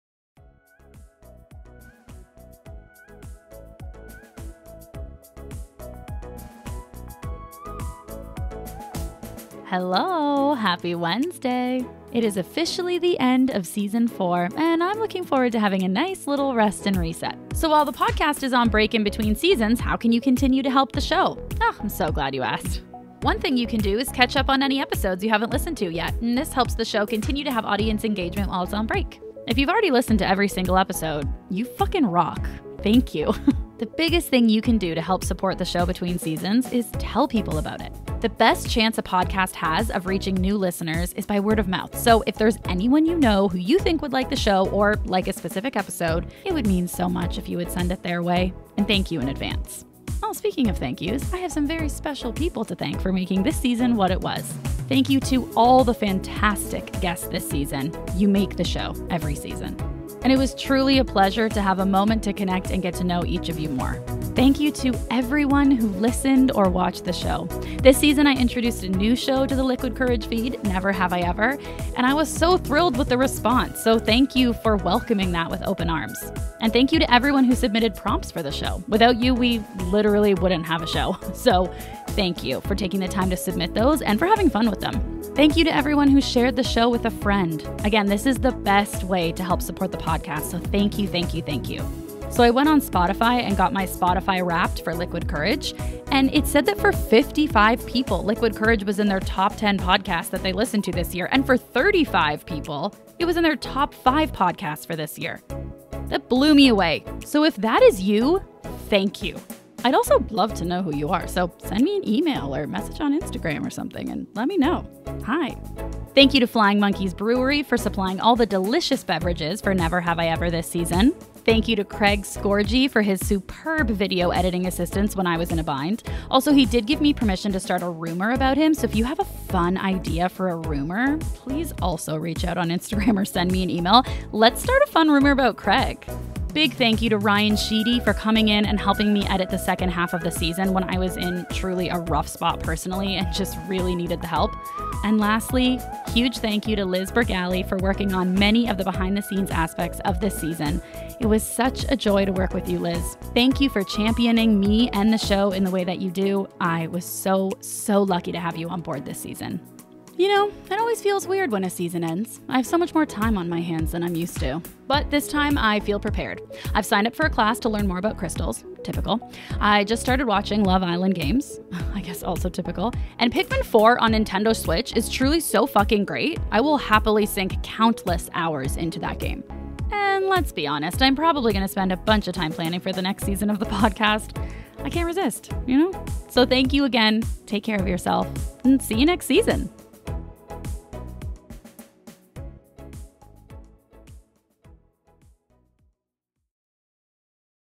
The Liquid Courage Podcast - See You Next Season!
It is officially the end of Season 4 – thank you for a fantastic season!!

…all the fantastic guests this season
…everyone who listened and watched the show
…everyone who submitted prompts for Never Have I Ever
…Flying Monkeys Brewery
…Craig Scorgie (video edit)
…Ryan Sheedy (audio editing)
…Liz Bragale (behind-the-scenes, superstar)

While the podcast is in between seasons, here are 2 ways you can continue to support the podcast:
1) Listen to any episodes you haven’t listened to yet – this helps the show continue to have audience engagement while on break.
2) The biggest help – Tell people about the show! The best chance a podcast has of reaching new listeners is by word-of-mouth, so if there’s anyone you know who might like this podcast or a specific episode, it would mean so much if you would send it their way (and thank you in advance).

So thank you again, take care of yourself, and see you next season! I already can’t wait :)